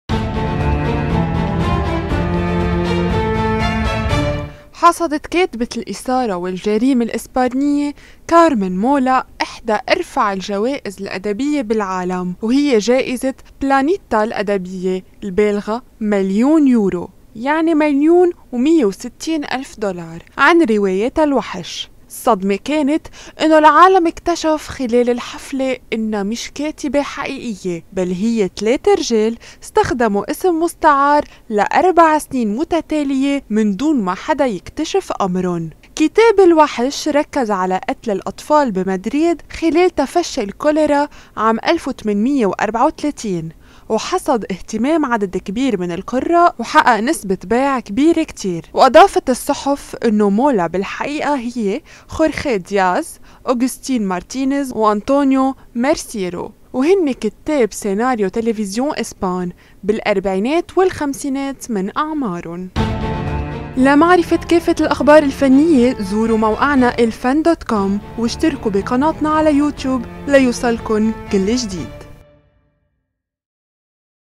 كاتبة شهيرة اتضح أنها 3 رجال في حدث صدم العالم
حصدت كاتبة الإثارة والجريمة الإسبانية كارمن مولا إحدى أرفع الجوائز الأدبية في العالم، وهي جائزة بلانيتا الأدبية البالغة مليون يورو (مليون و160 ألف دولار) عن روايتها "الوحش" .  الصدمة كانت ان  العالم اكتشف خلال الحفلة أنها ليست كاتبة حقيقية، بل هي 3 رجال قد استخدموا اسم مستعار لأربعة سنين متتالية من دون ما حدا يكتشف أمرن.

Copyright : ElFann.com